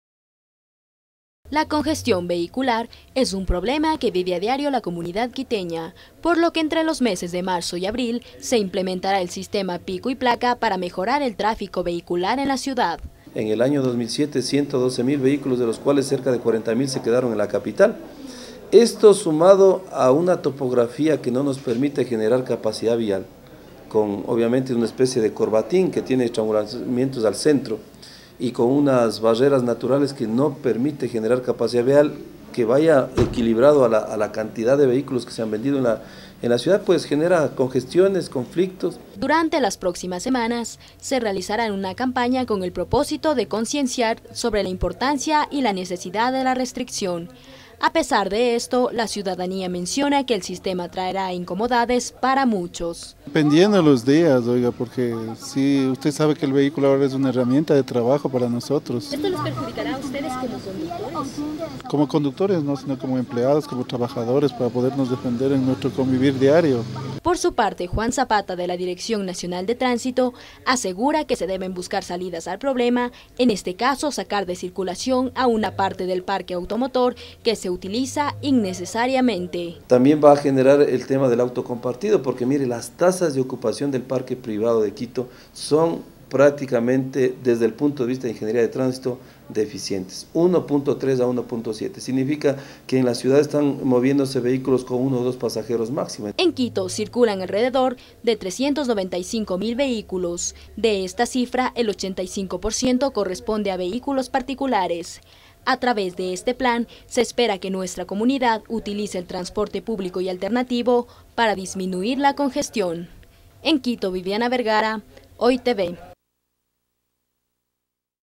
Pico y placa se apliacaría en Quito
La aplicación de la restricción vehicular en Quito no será inmediata, dijo el pasado lunes, 11 de enero, alcalde Augusto Barrera al dar a conocer el proyecto. HOY TV entrevistó al vocero de la Dirección Nacional de Tránsito, Juan Zapata.
Dirección: Julio Torres
Cámara y Edición: Cristian Flores
Reportera: Viviana Vergara